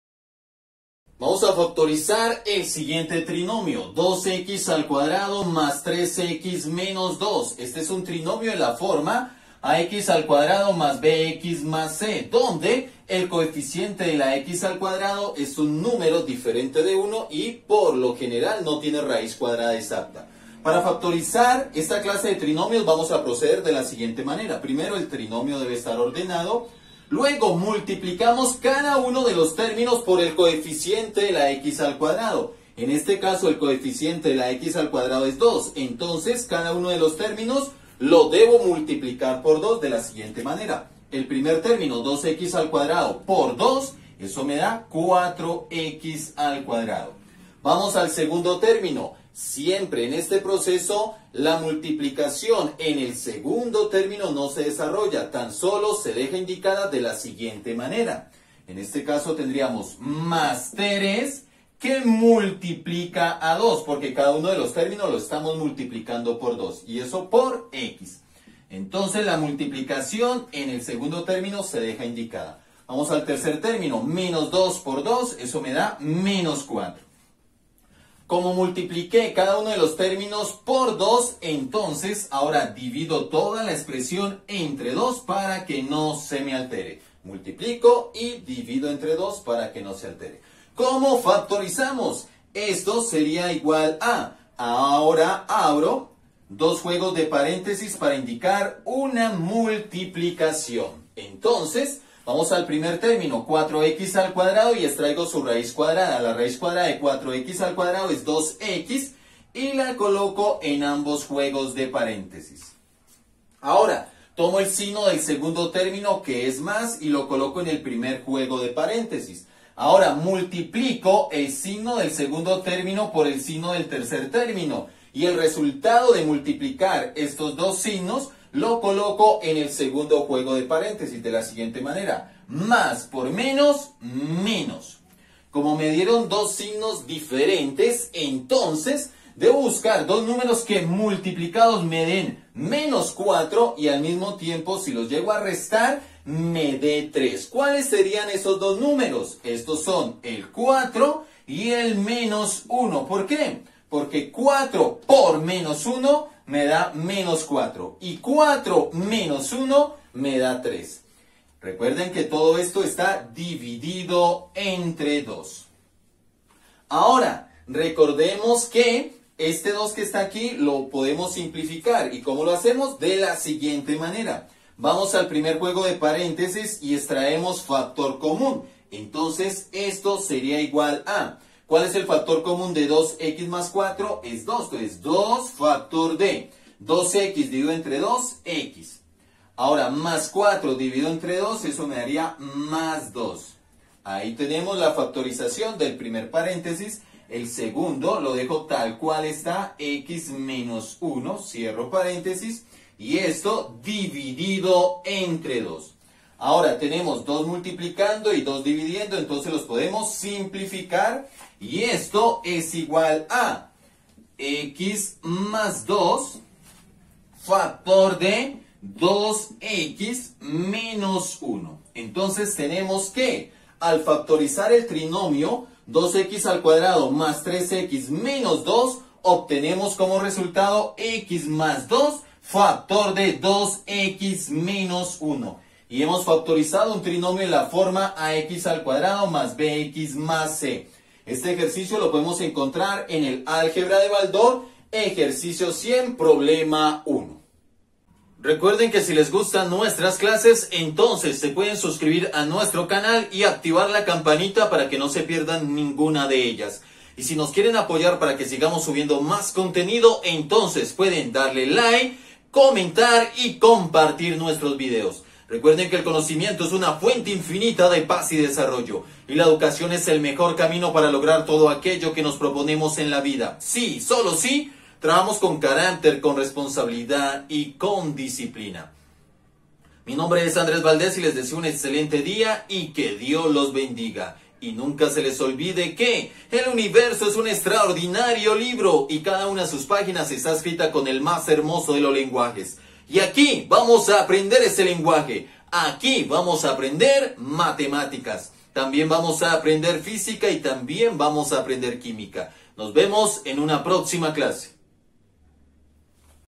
FACTORIZACIÓN DE UN TRINOMIO ax2+bx+c / Álgebra de Baldor ejercicio 100 problema 1 / 2x2+3x-2
FACTORIZACIÓN DE UN TRINOMIO ax2+bx+c
Álgebra de Baldor ejercicio 100 problema 1
2x2+3x-2
Paso a paso se factoriza un trinomio de la forma ax2+bx+c
PROFESOR ANDRÉS VALDEZ CORTÉS